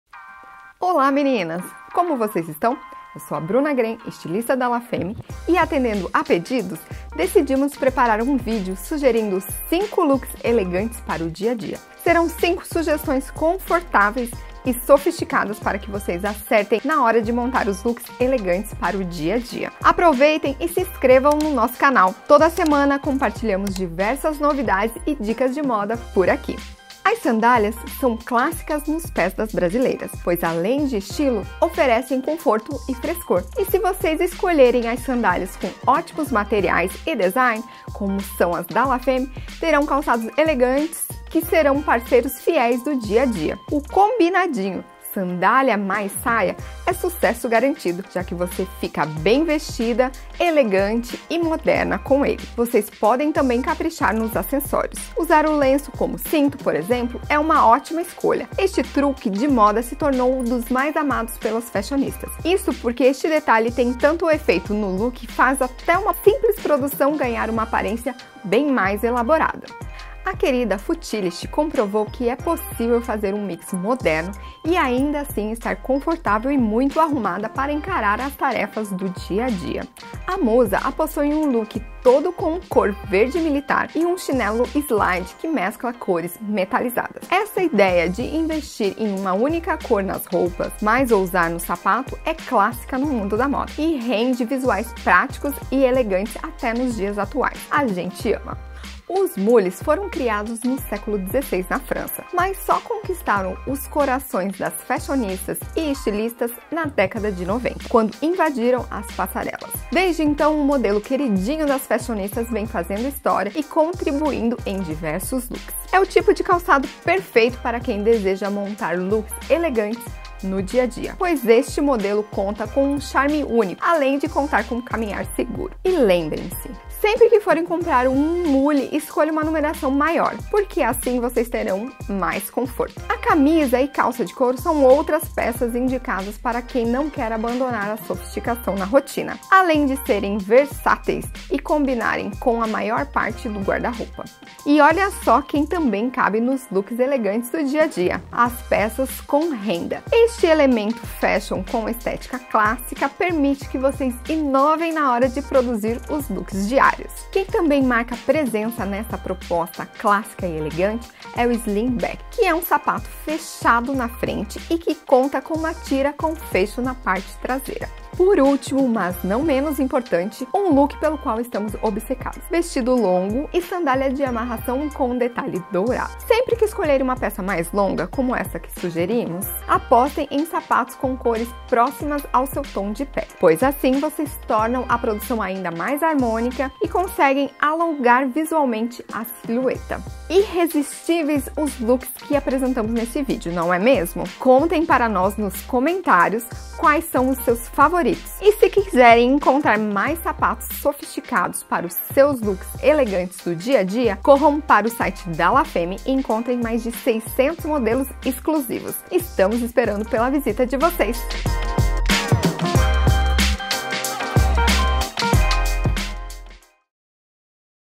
05 looks elegantes para o dia a dia - La Femme Calçados
A estilista da La Femme, Bruna Grein, trouxe 05 sugestões de looks elegantes para o dia a dia. As combinações são criativas, elegantes e atemporal. Aperte o play para conferir!

Você pode conhecer mais sobre todos os sapatos citados neste vídeo acessando

CAPÍTULOS:
0:00 - Introdução
00:44 - Look elegante com sandália + saia
01:20 - Look elegante verde militar com chinelo slide
02:05 - Dica para escolher o tamanho certo do mule
02:13 - Look versátil com mule
02:28 - Look elegante com renda e slingback
02:52 - Look com vestido longo e sandália amarração
03:18 - Onde comprar as flats